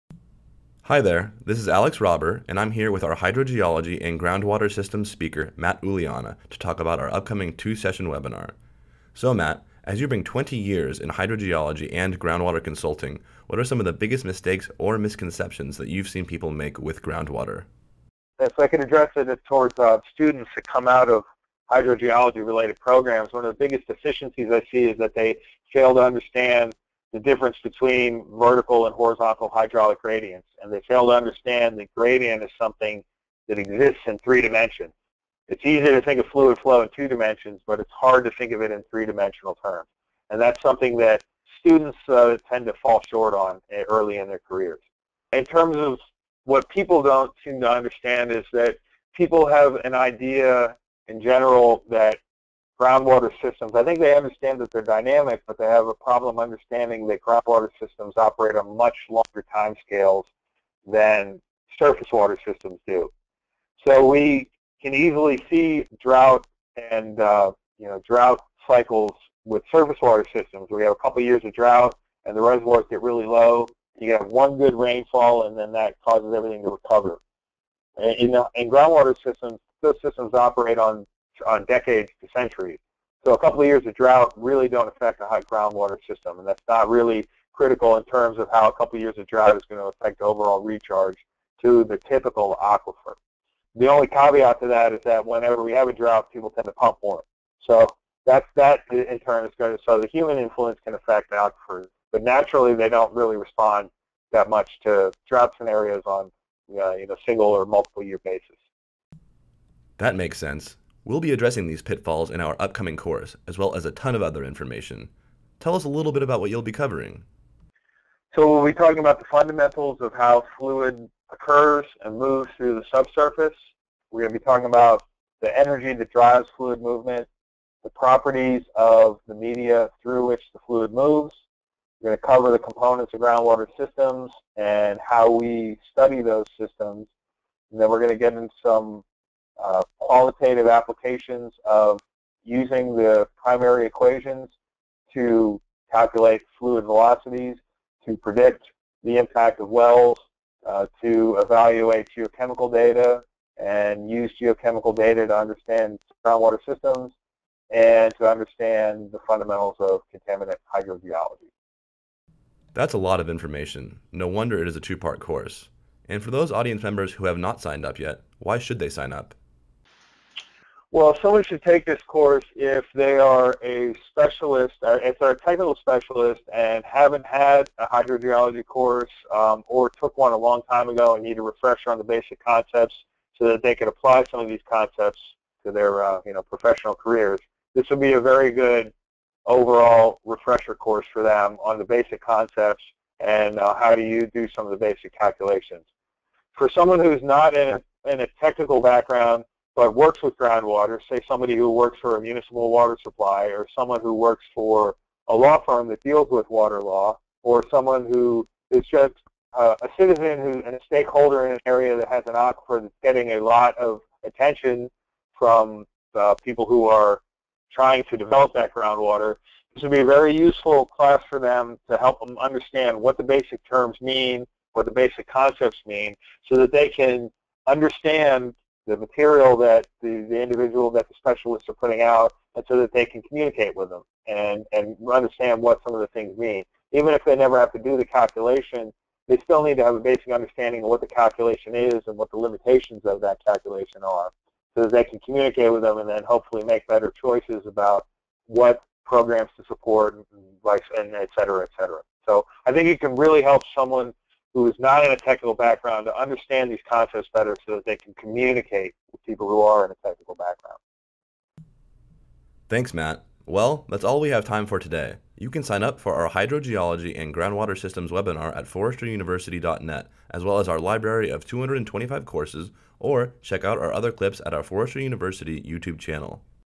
Hydrogeology 101 - The Biggest Mistakes
If you have high groundwater, unstable soil, or shallow bedrock, when the winter storms head your way…you could be in trouble. How well do you know how to measure and calculate the groundwater at your site? AND how confident are you in predicting the erosion impact at your site when winter floods your groundwater? Stay ahead of the storms! Learn to measure, calculate, and predict your groundwater impacts.